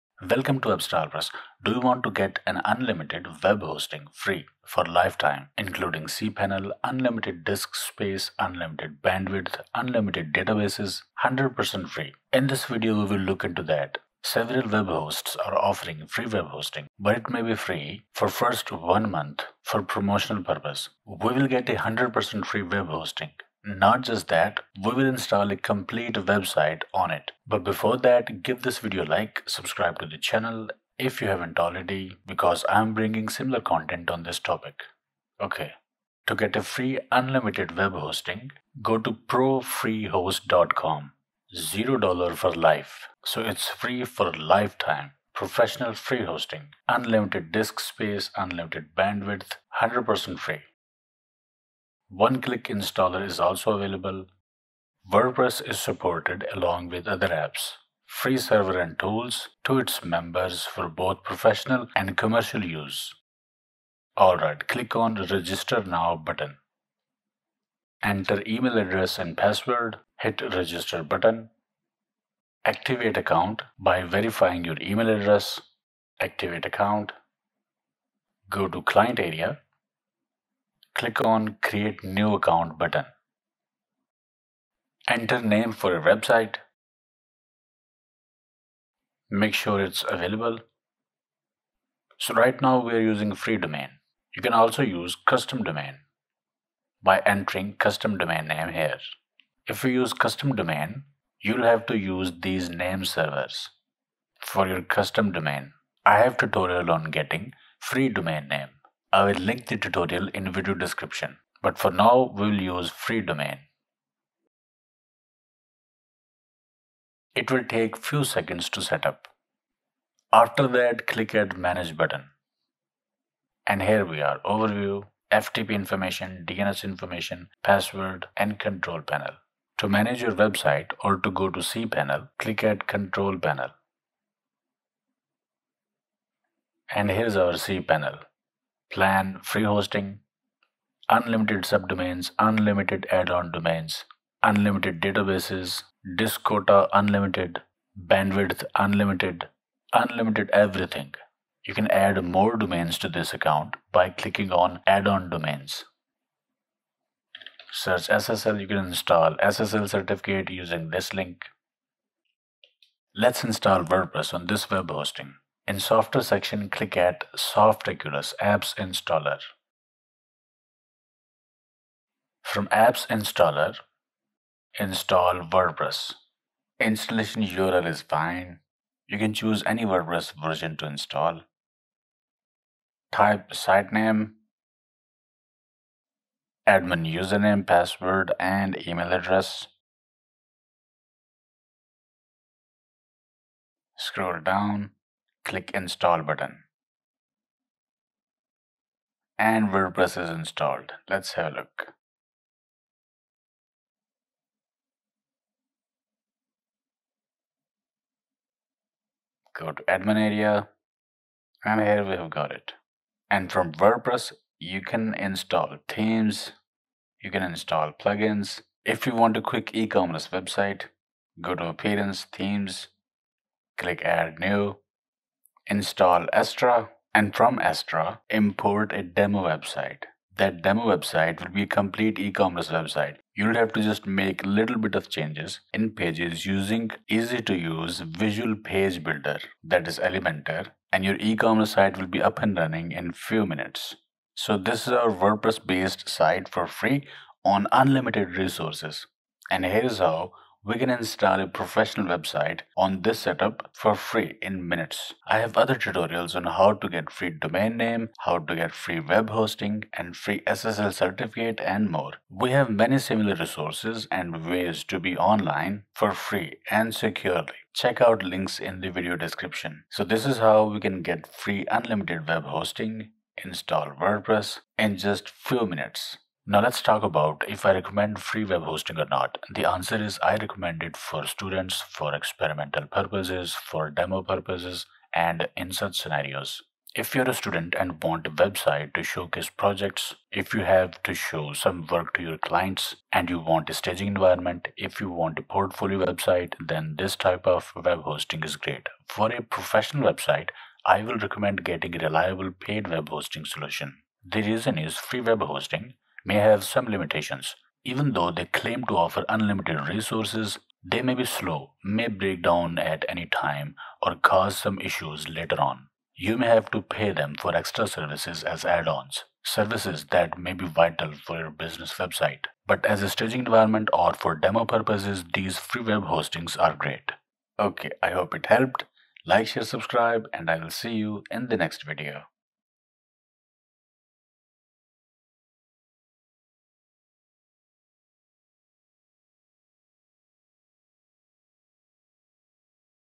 How to Get Unlimited Free Web Hosting for Lifetime | Free Web Hosting for Wordpress Website
How to get unlimited free web hosting for lifetime? Get free web hosting for wordpress website. Do you want to get an unlimited web hosting free for lifetime, Including Cpanel, unlimited diskspace, unlimited bandwidth, unlimited databases, 100% free, without ads or limitations? We will get a 100% free web hosting. Not just that, we will install a WordPress site on it. So video will show you how can you get free WordPress hosting.

Go to the profreehost site mentioned in the video. It's 100% free unlimited web hosting. One click installer is also available so that you can install wordpress in one click. Wordpress is supported along with other apps. Good for both personal and commercial use.

Register a free account with them
Go to client area
Create account
It will take few seconds to setup
There you will see account overview, FTP, DNS, Password settings
Click on 'Control Panel'
This is Cpanel based control panel

You can add unlimited add-on domains and apply free ssl certificate as well.
To install wordpress, click at apps installer and choose wordpress
WordPress will be installed
Install Astra theme and import demo site to quickly create full working demo site. After that you can easily change it by using Elementor page builder.

This is wordpress based site for free on an unlimited resource. Here is how we can install a professional website on this setup for free in minutes. We have many similar resources and ways to be online for free and securely.

I have other tutorials on:
How to get free domain name
How to get free web hosting
How to get free SSL Certificate

So this is how we can get free unlimited web hosting, install wordpress and connect it to a website
in just minutes.

Now let's talk about if I recommend free web hosting or not. The answer is, I recommend it for students, for experimental purposes, for demo purposes, and such scenarios. If you are a student and want a website to showcase projects, if you have to show some work to your client and you want a staging environment, if you want a portfolio website, then this type of web hosting is great.

For a professional website, I will recommend getting a reliable paid web hosting solution. The reason is, free web hosting may have some limitations, even though they claim to offer unlimited resources, they may be slow, may break down at any time, or cause some issues later on. You may have to pay them for extra services as add-ons - services that may be vital for your business website. But as a staging environment or for demo purposes, these free web hostings are great.